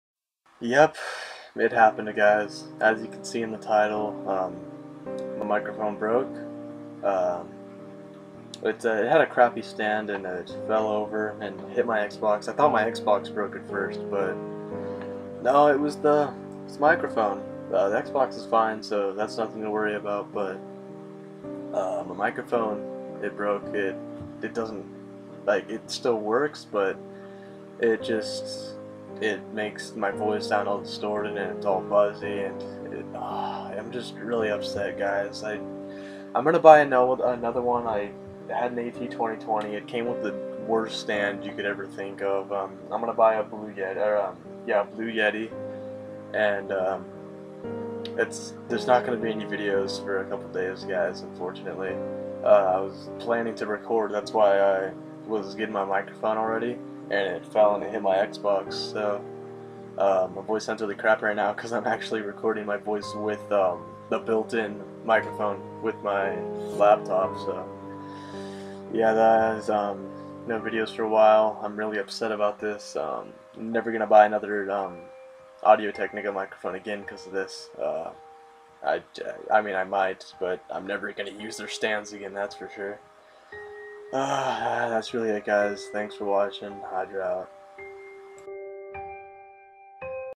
My Microphone Broke :(
So my mic had a crappy stand and it fell of my desk and hit my Xbox. My Xbox made a weird noise and thought that my Xbox broke but its fine. Ill be getting a new microphone within the next 5 days. I will hopefully be getting one by August 9th. Sorry for the inconvenience